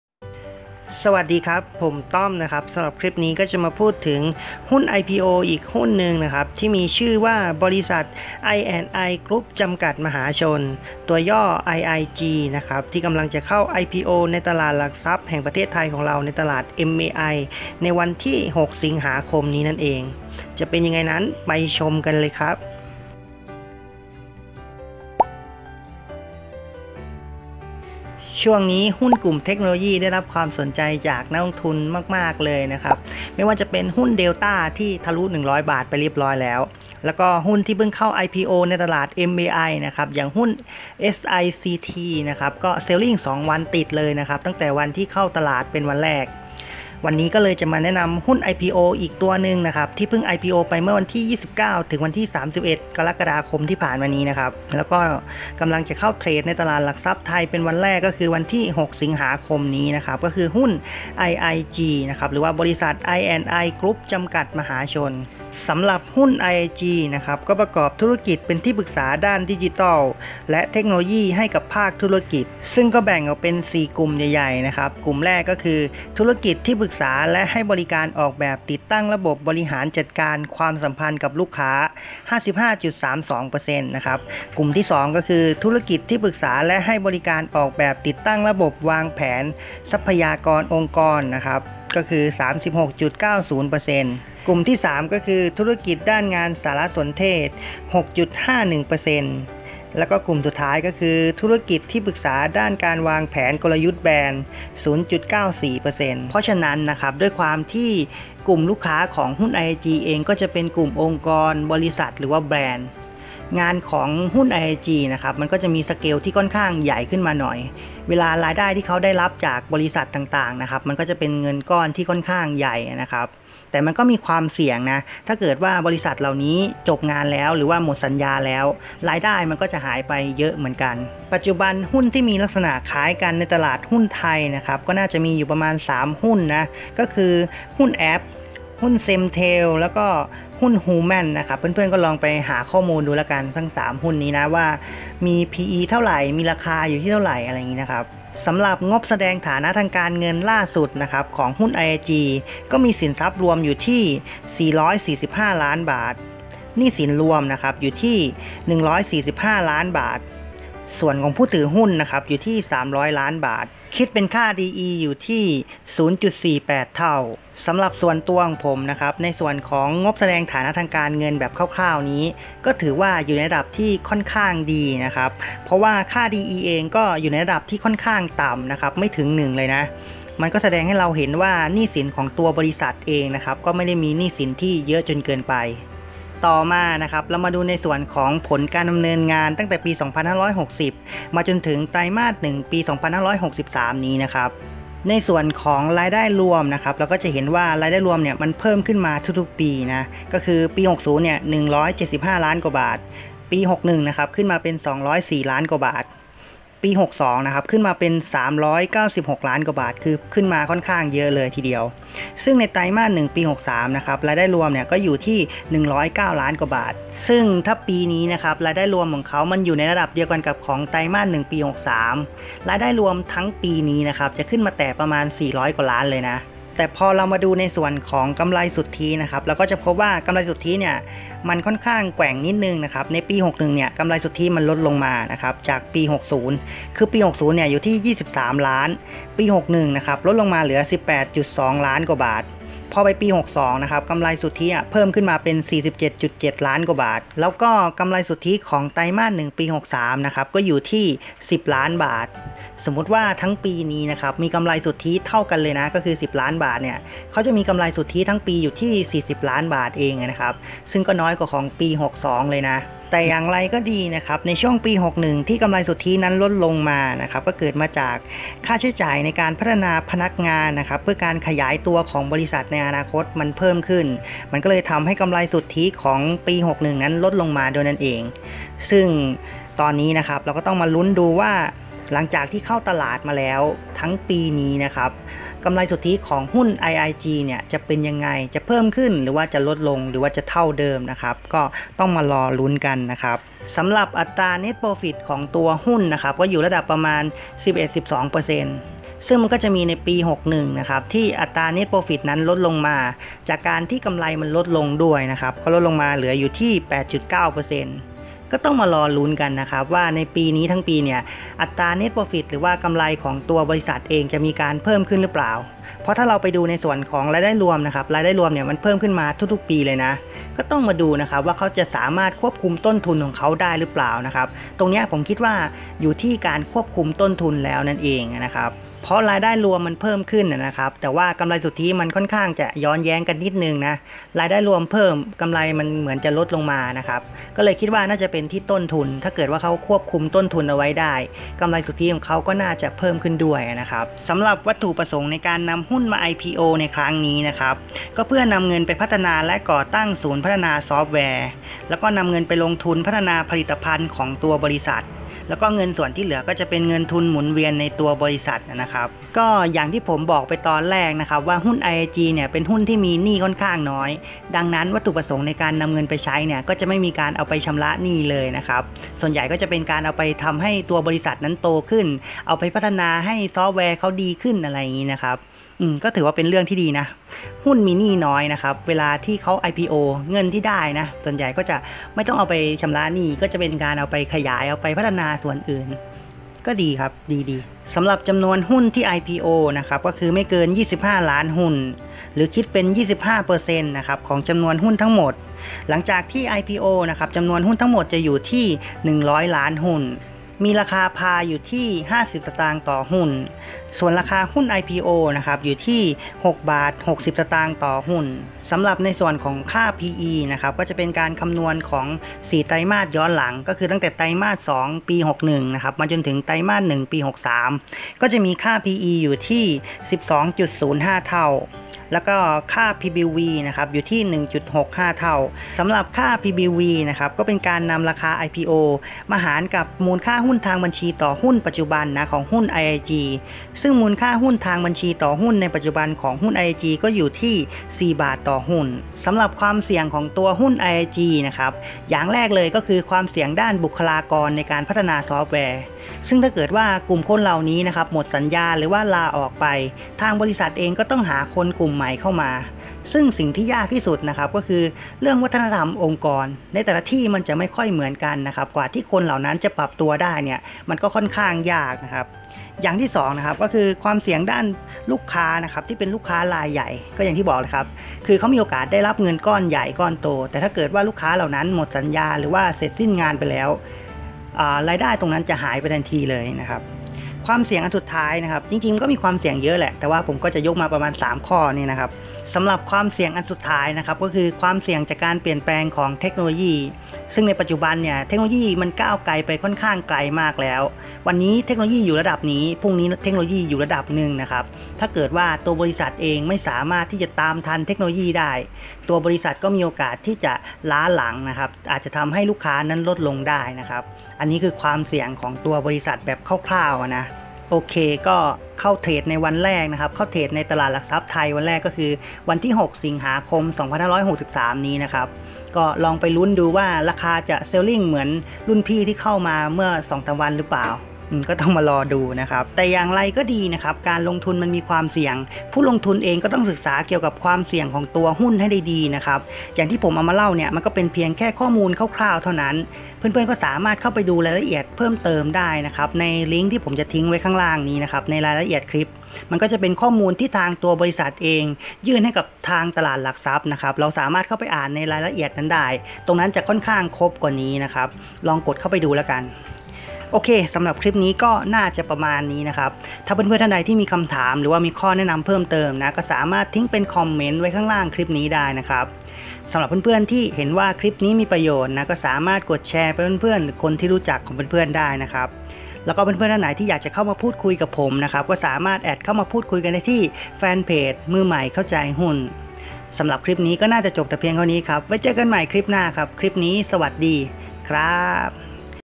EP.382 IPO หุ้น IIG บมจ.ไอแอนด์ไอกรุ๊ป [ นักลงทุนมือใหม่ ]
หุ้นกลุ่มเทคโนโลยีอีกหุ้นในตลาด mai อย่าง iig หรือ บมจ.ไอแอนดไอกรุ๊ป
จะน่าสนใจขนาดไหนนั้นมาดูกันครับ

Kick worathepnitinan
rakesh Madarasmi
Boonthawee Tanmaneelert
Som Cchai
Songpol Medical pranks

ลิ้งค์ดาวโหลดสติ๊กเกอร์ไลน์